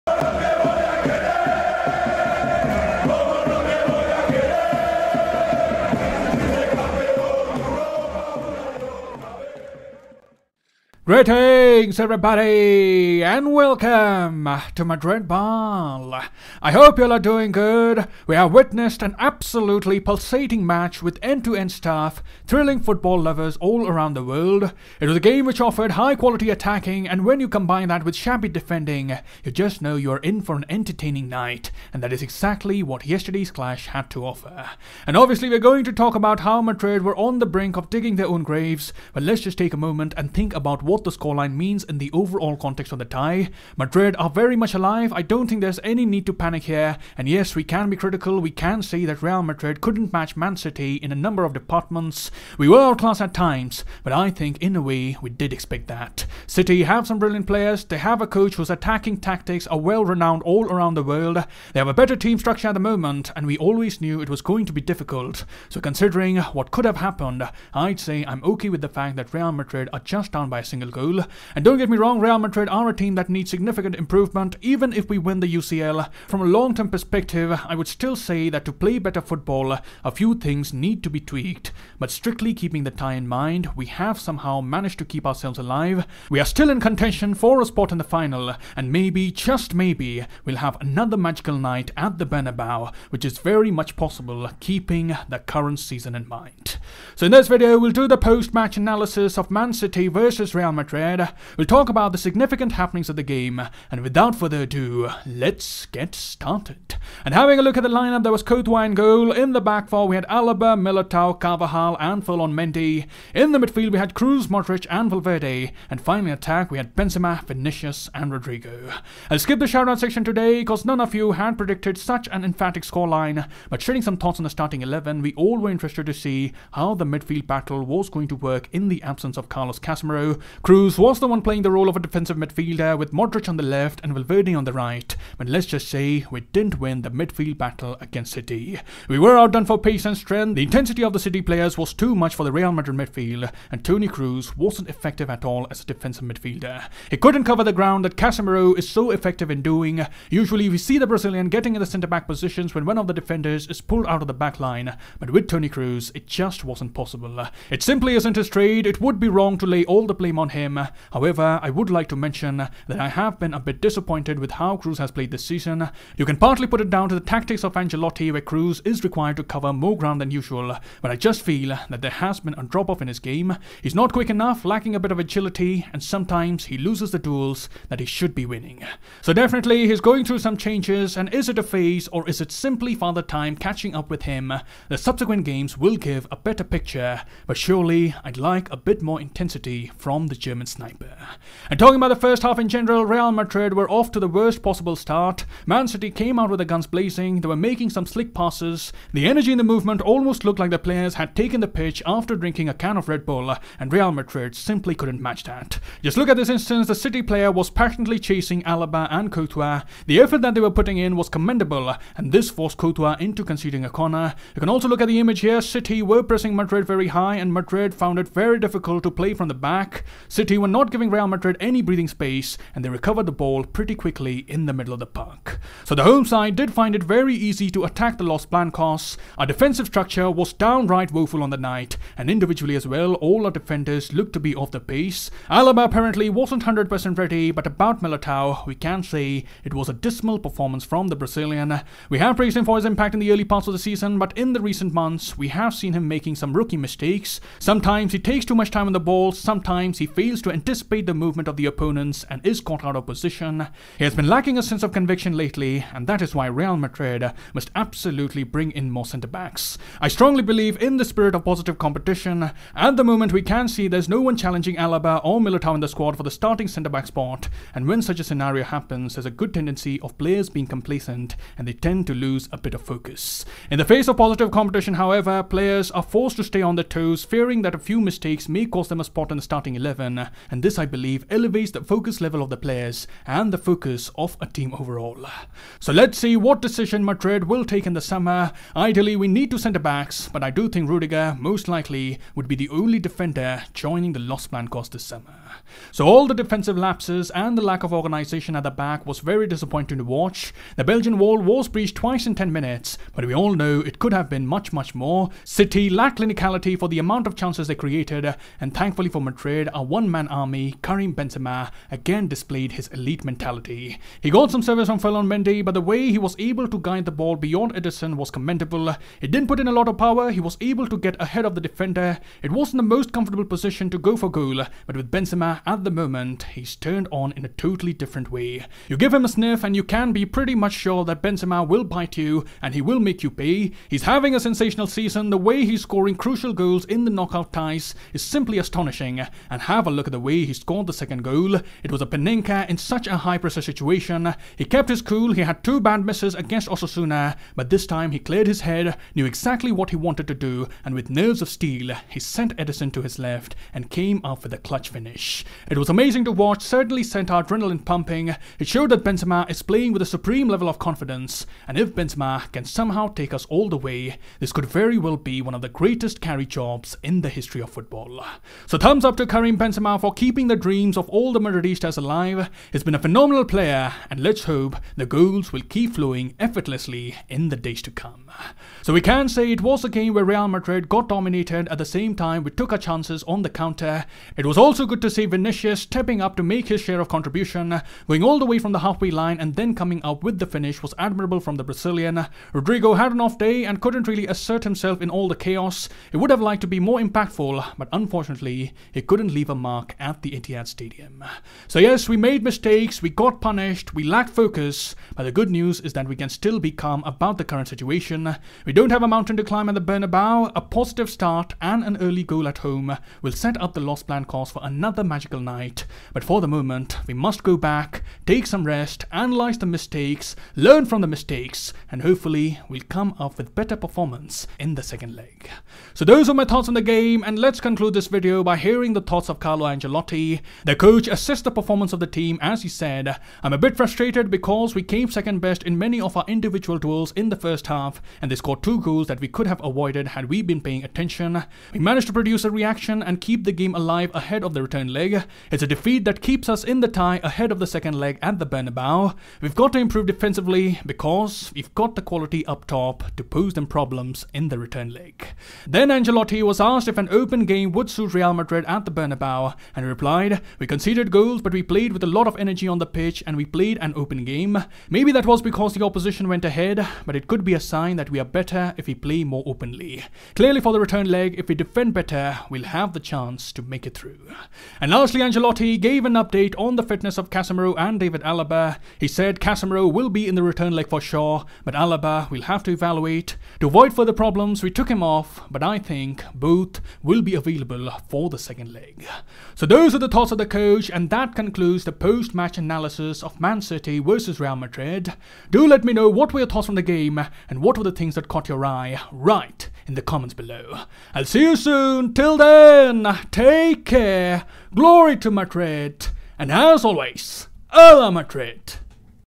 Man City vs Real Madrid 4-3 post match analysis !!
This is the post match analysis of Man City vs Real Madrid
Real Madrid played Man City in the first leg of the semi finals of the UEFA Champions League. It was a game of end to end football. City largely dominated and were able to score four goals but Real Madrid as well came up with a response through goals from Karim Benzema and Vinicius Junior. Real Madrid only trail by a single goal but they would be hoping that the magic of the Bernabeu will come in handy when they take on City in the 2nd leg.